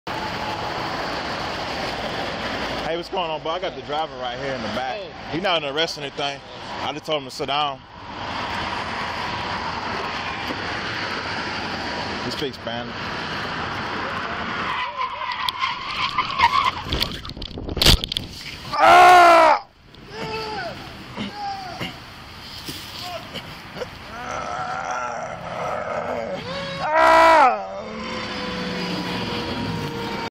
Officer Carter, DWI Taskforce, Body Camera Footage | Houston Police
Body camera footage from HPD Officer Carter, DWI Taskforce, who was hit by a drunk driver and survived an 18 foot fall off the freeway on July 28, 2017.

Produced by:

Public Affairs Division
Houston Police Department